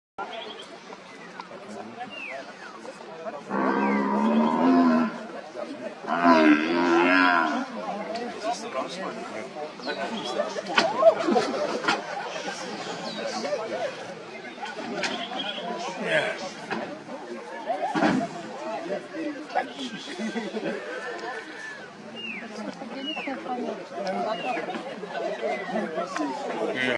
Buffalo Rescue Gone Wrong
Buffalo got stuck at a waterhole in Kruger National Park, rangers rescued it but he did not like the attention at all